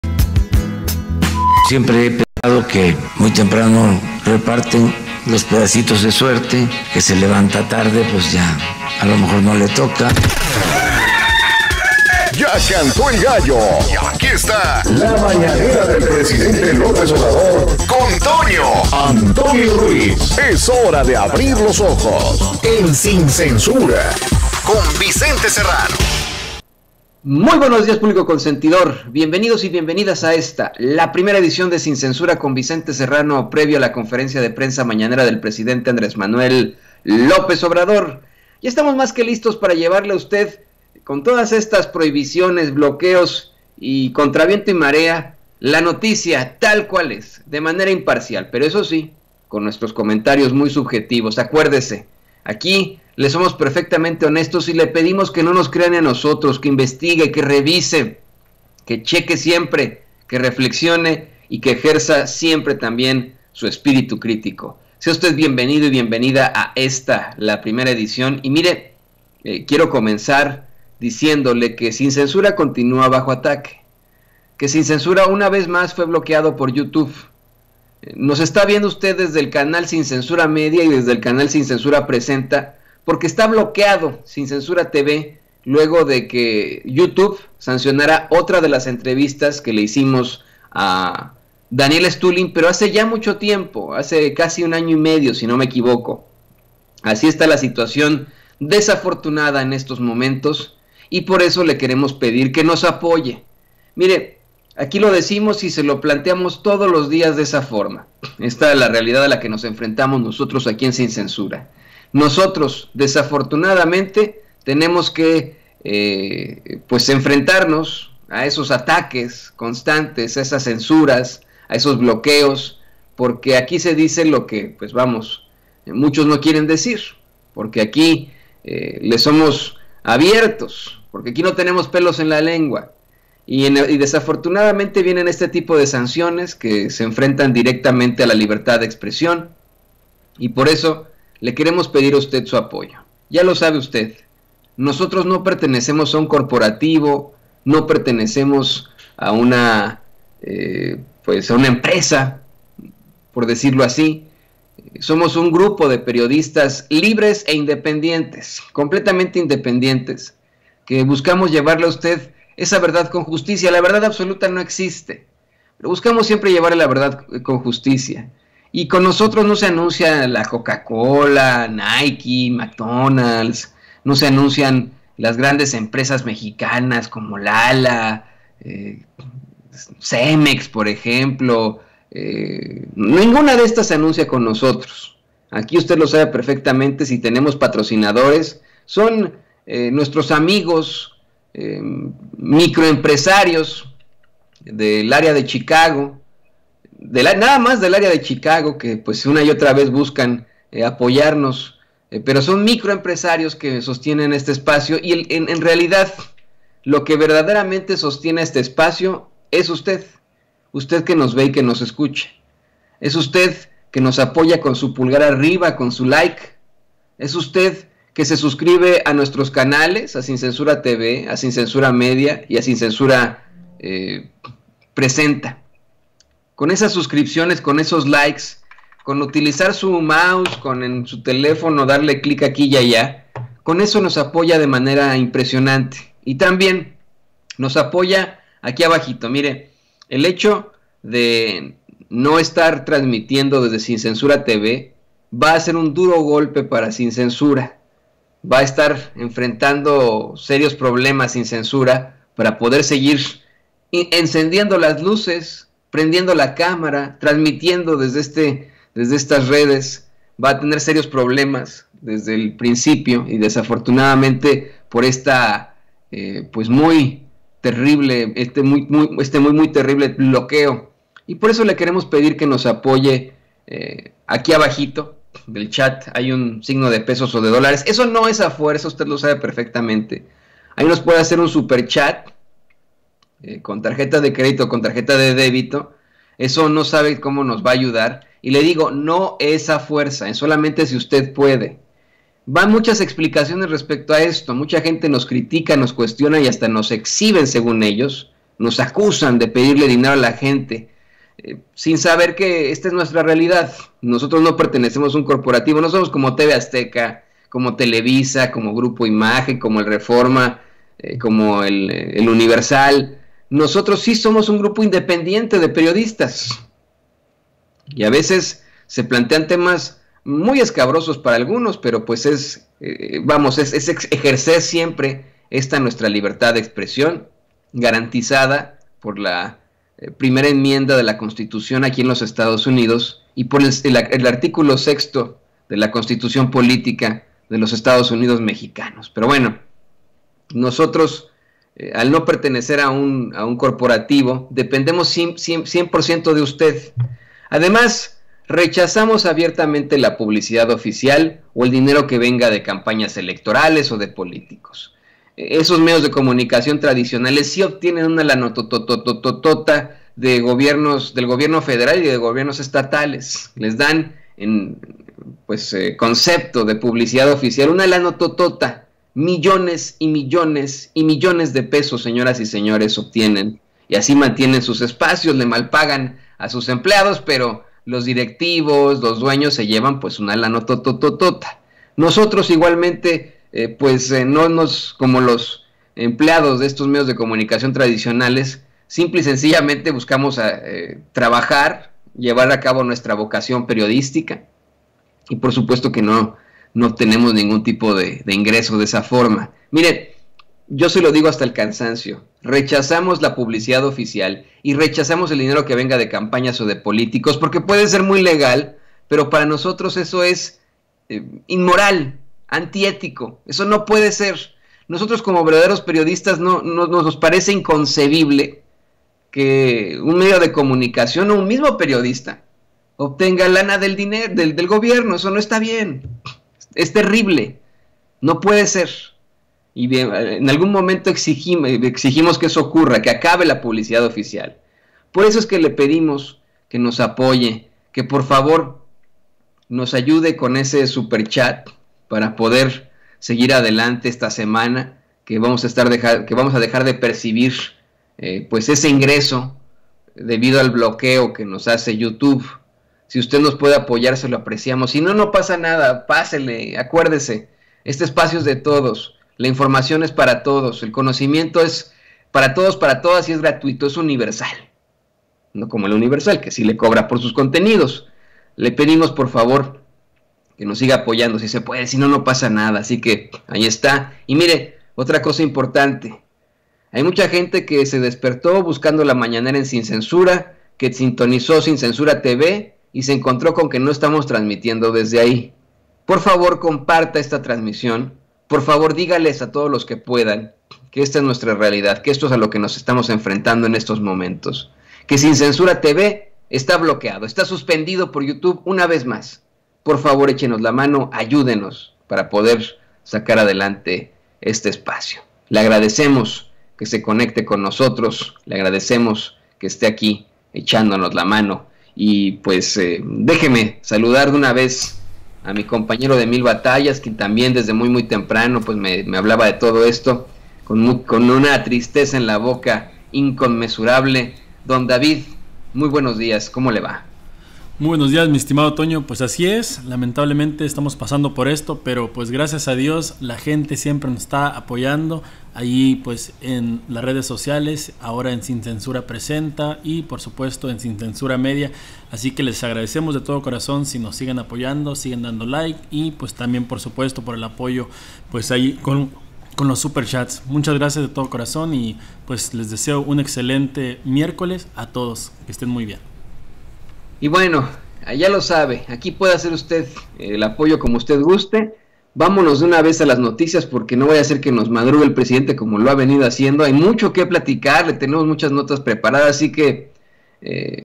YOUTUBE NOS VOLVIÓ A BLOQUEAR… AHORA SUSPENDEN SIN CENSURA TV ¡POR FAVOR AYÚDENOS!
YouTube nos volvió a bloquear… Ahora suspendieron Sin Censura TV por otra de las entrevistas con Daniel Estulin ¡Por favor ayúdenos a mantener este espacio a flote!